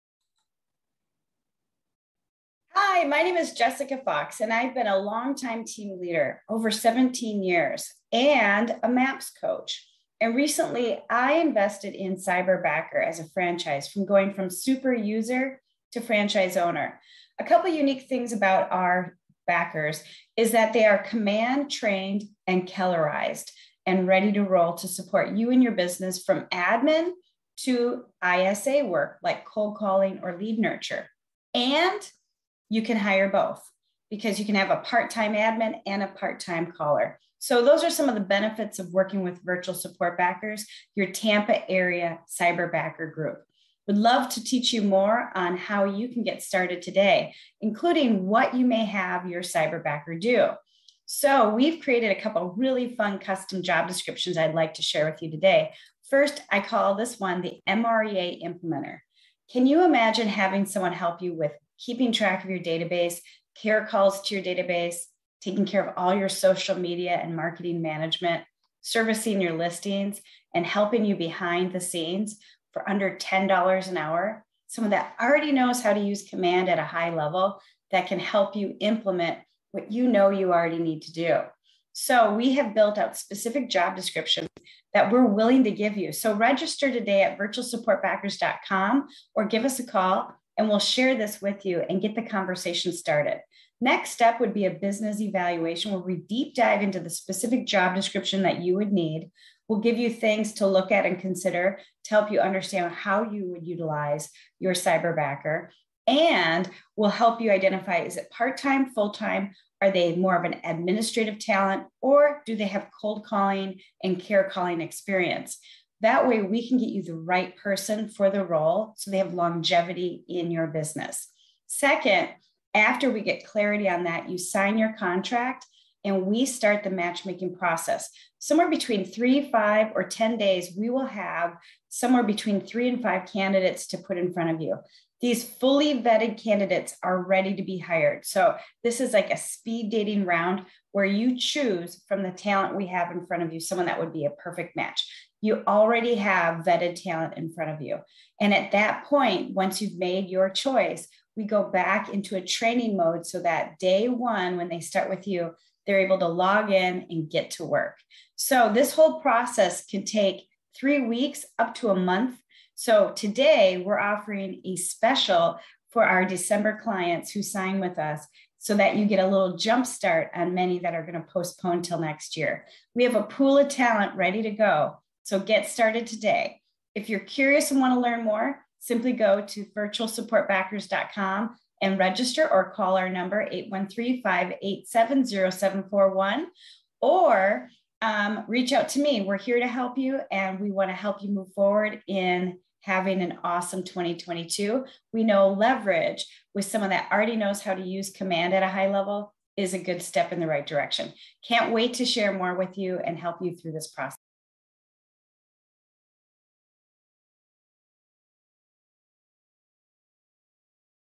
Why Virtual Support Backers powered by Cyber Backer over other companies...
Jessica Fox shares what makes Virtual Support Backers powered by Cyberbacker SPECIAL.

Jessica also shares the most popular job descriptions!

If you are outside of our franchise area in Florida, feel free to register and we will connect you with your local franchise owner.